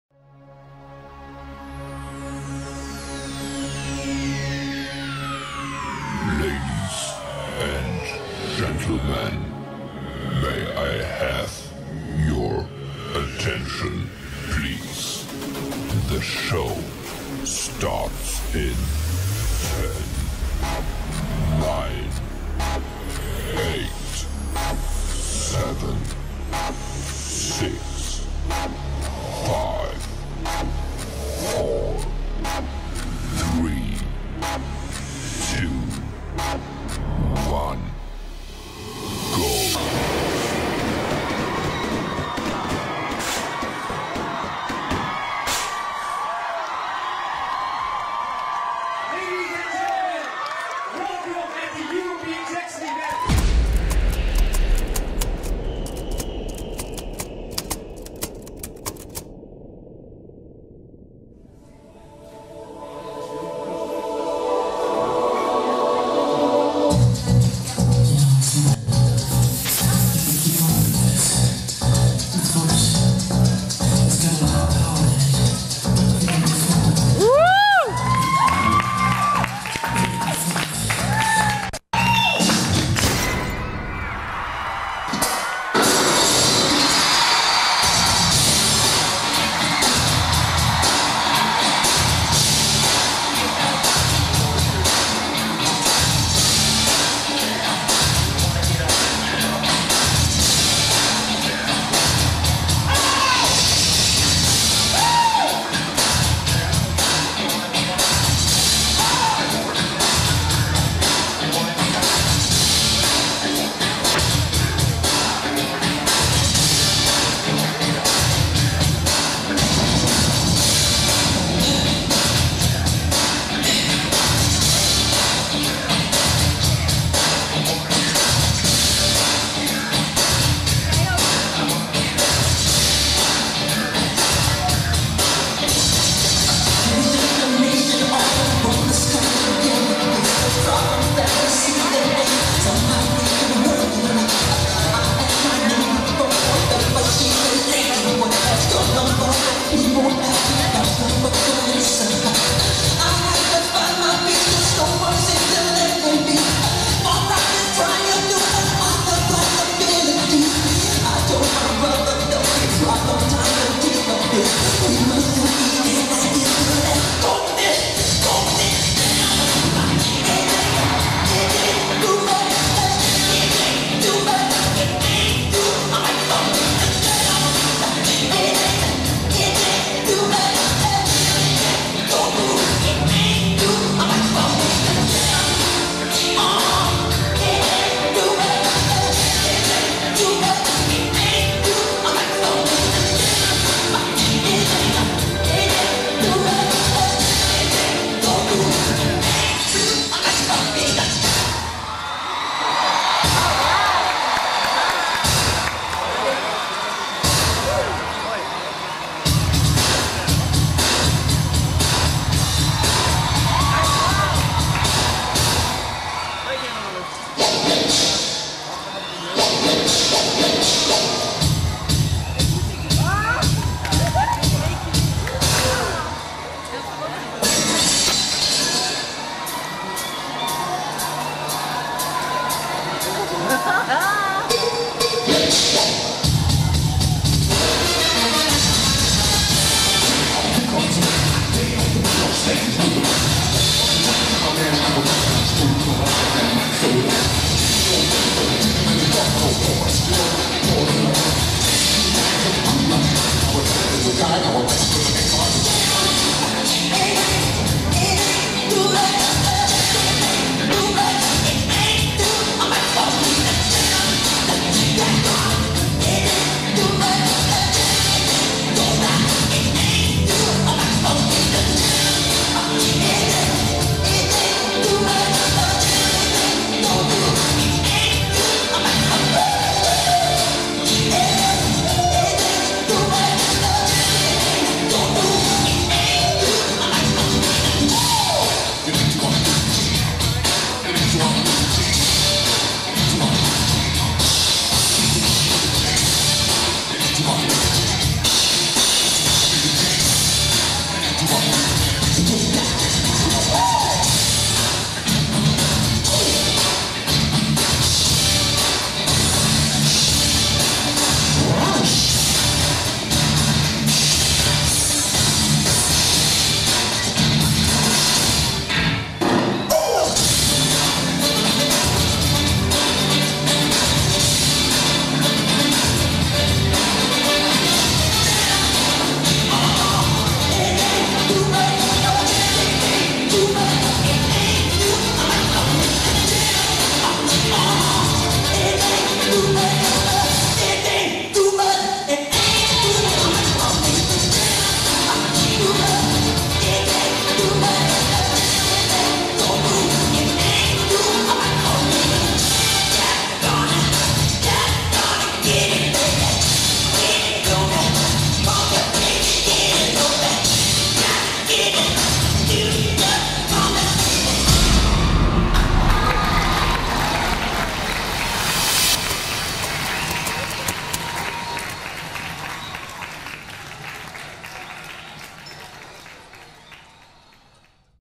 Jackson Event Memories Part 17: Milly Magic 2001
Thanks Olaf & Syvano for the footage

The first event we did was in 2001, the last in 2014 so a lot of memories. Last year I digitized all my VHS/ DV tapes and I found a lot :) A lot of unseen footage: Messages from Michael, backstage footage, tibute video's, rehearsals, commercial shoots etc. At the end all we have is memories. and I would like to share it with you, Enjoy :)